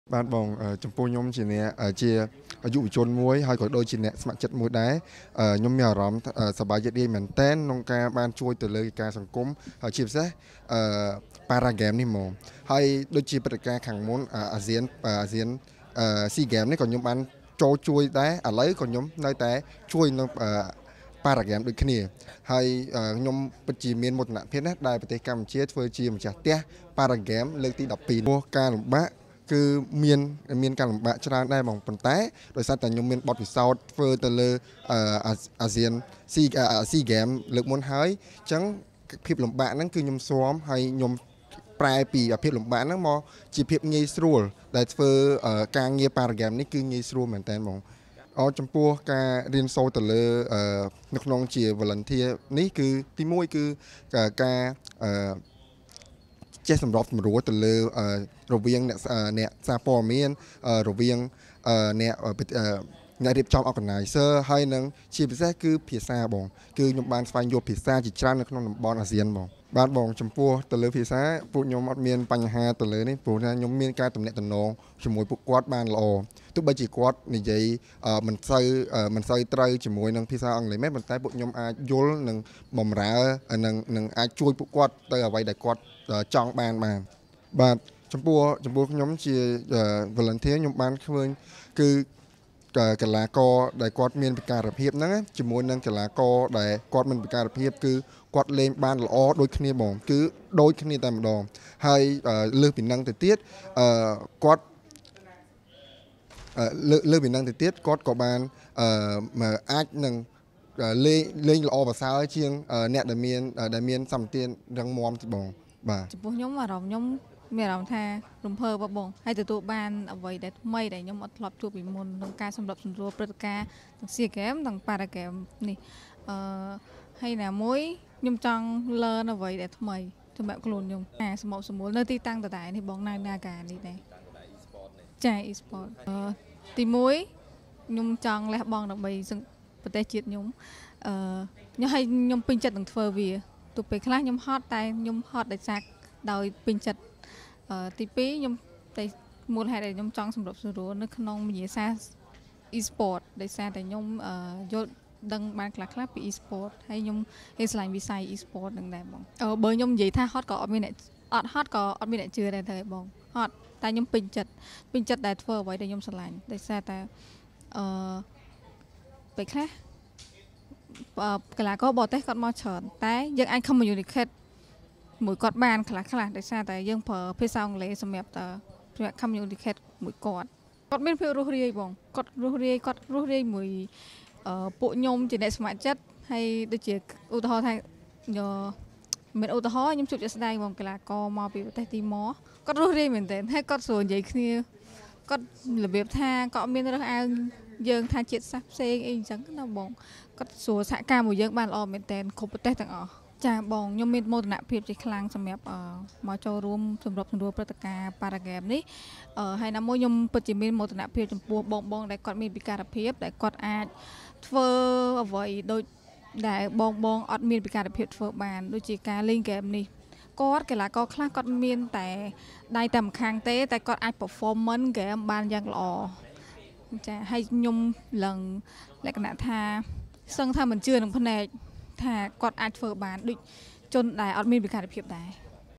[CAMBODIA 2023] : ក្រុមយុវជនស្ម័គ្រចិត្ត សម្របសម្រួលក្នុងកីឡា Esport បង្ហាញ ក្តីសប្បាយរីករាយ
[CAMBODIA 2023] : ក្រុមយុវជនស្ម័គ្រចិត្ត សម្របសម្រួលក្នុងកីឡា Esport បង្ហាញ ក្តីសប្បាយរីករាយ ជាមួយការងារនេះ ដោយពួកគាត់បានទទួលមេរៀនថ្មី បទពិសោធន៍ថ្មីបន្ថែមក្នុងកីឡាអាស៊ានប៉ារ៉ាហ្គេមលើកទី១២នេះ ខណះកាលពីព្រឹត្តិការណ៍កីឡាស៊ីហ្គេមលើកទី៣២ ពួកគាត់ក៏បានចូលរួមជាមួយសកម្មភាពការងារជាអ្នកសម្របសម្រួលម្តងរួចហើយ ៕